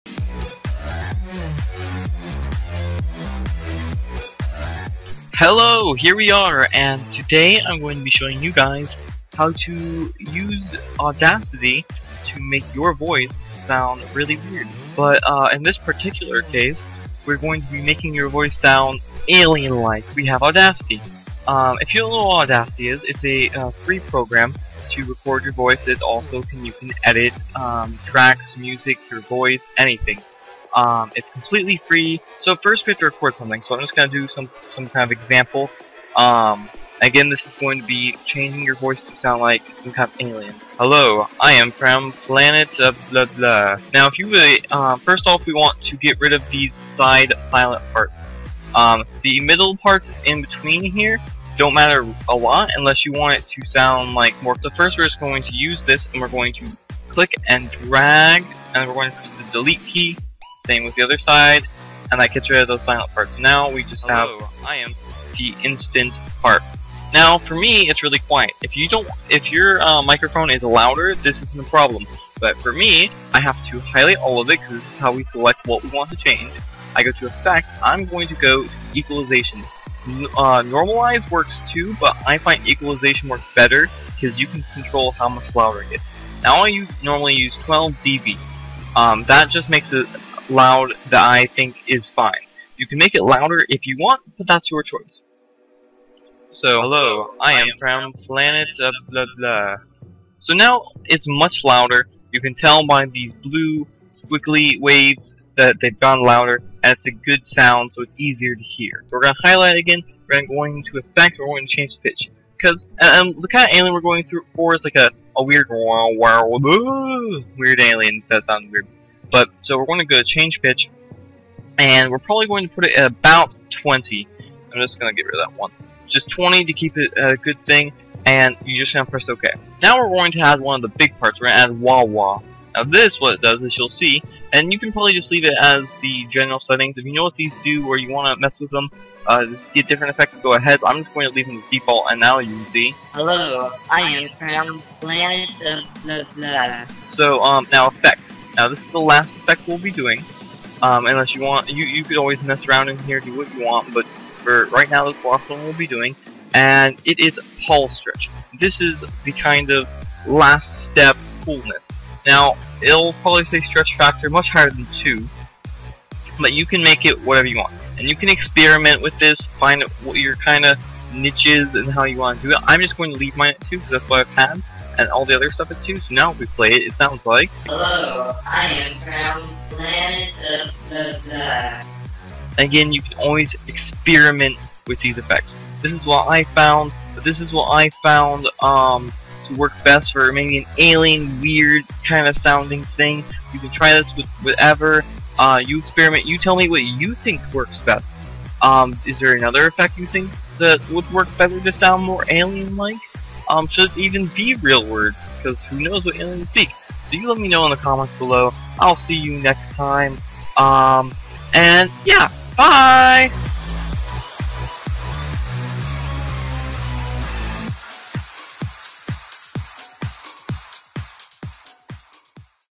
How To Make Your Voice Sound Like an Alien (With Audacity)
Cory shows you some neat tips and tricks to sound alien like using Audacity!